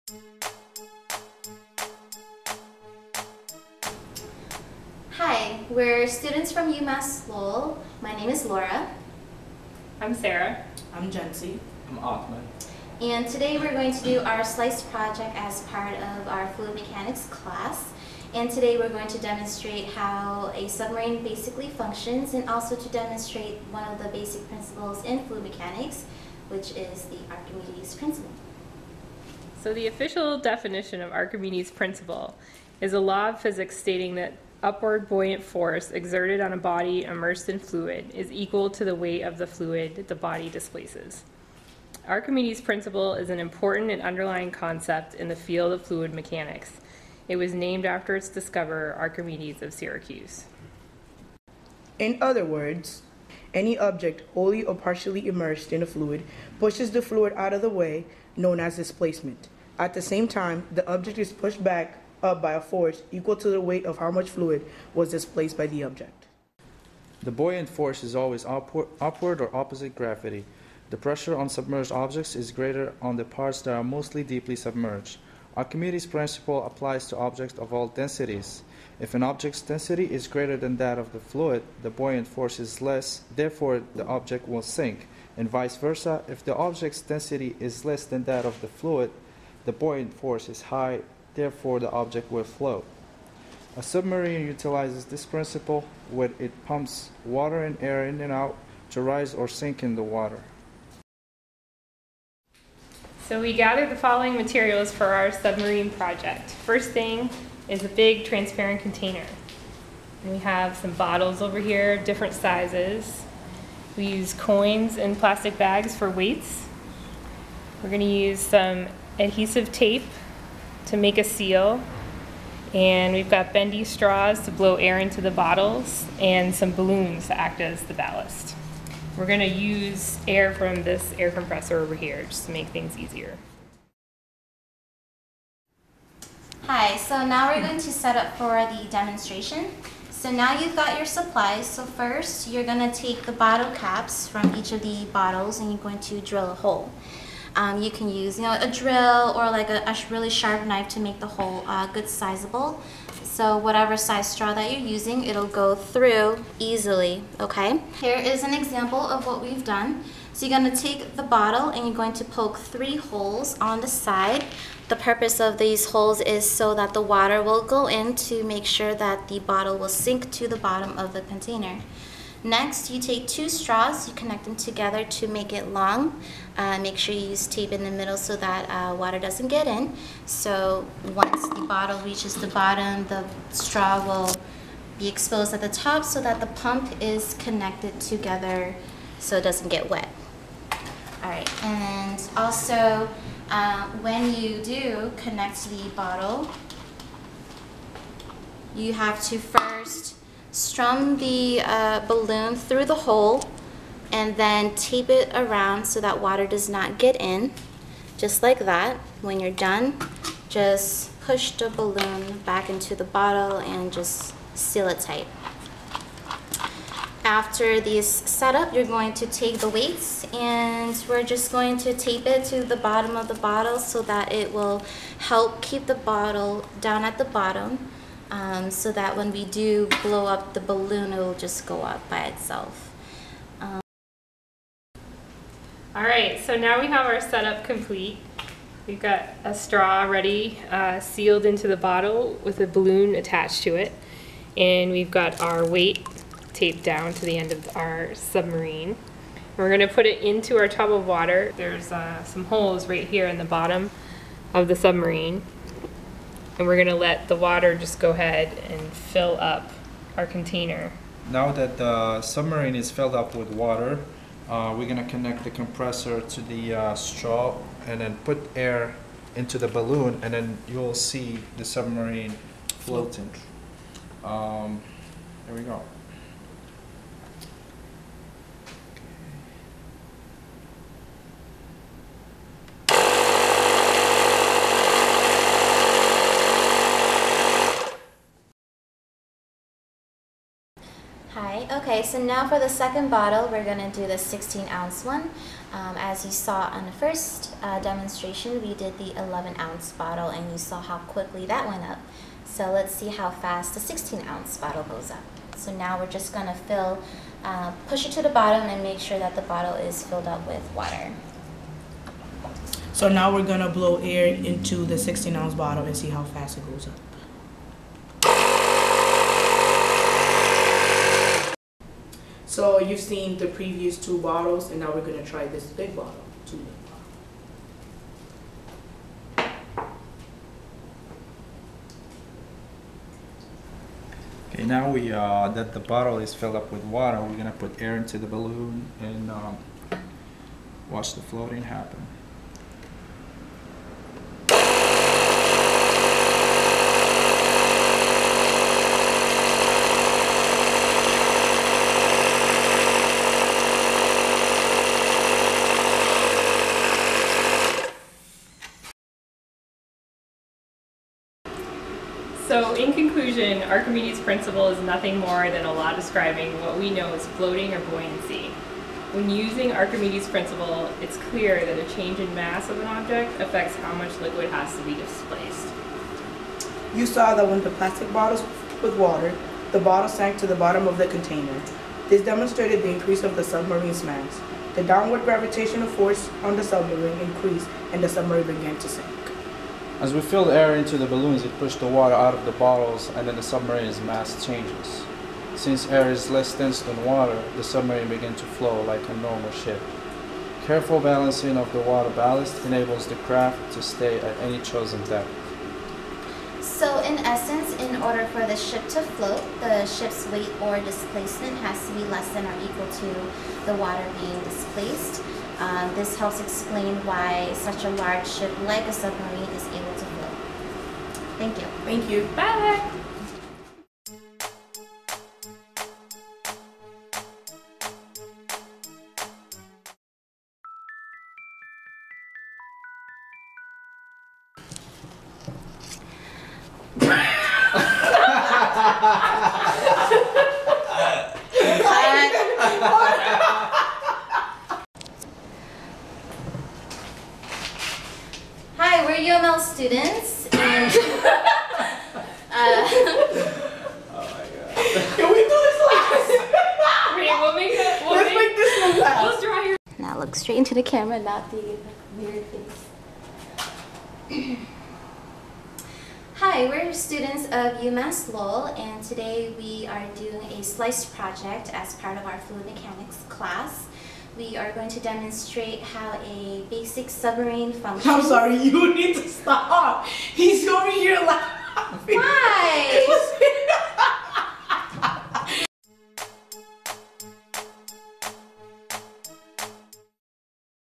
SLICE Project - Fluid Mechanics - Archimedes Principle - Bottle Submarine.mpg
Class project
High School Level
With bloopers